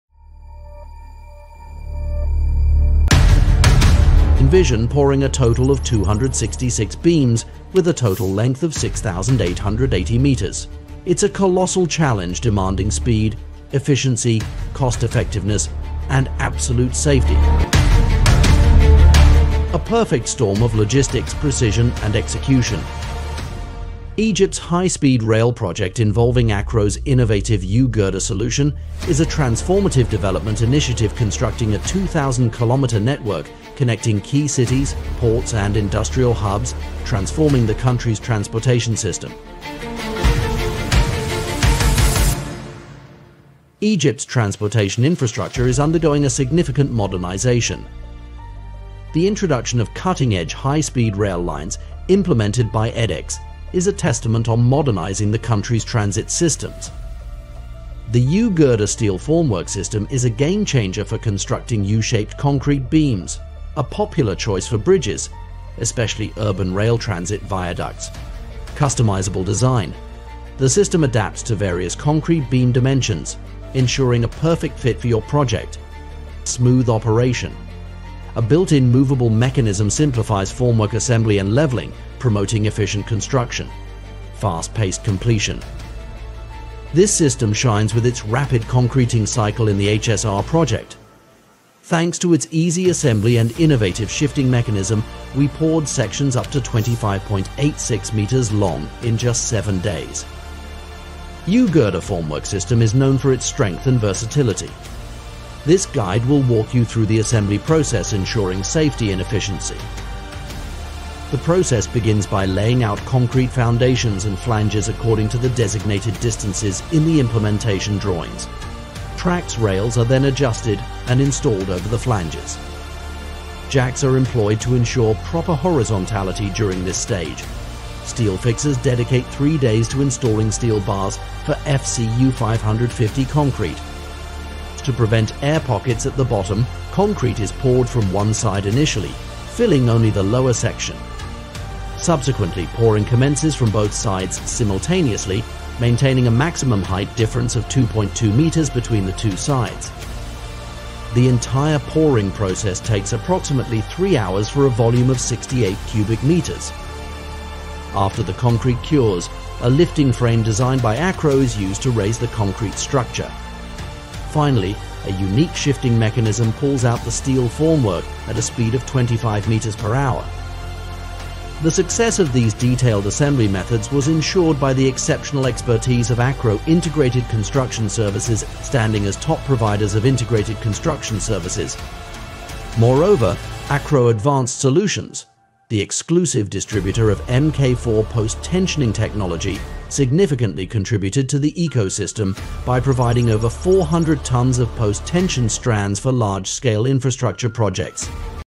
المسار العلوي للقطار الكهربي السريع داخل مدينة اكتوبر وتركيب الكمرات الخرسانية
شغل الترجمة CC واستمتع بالمعلومات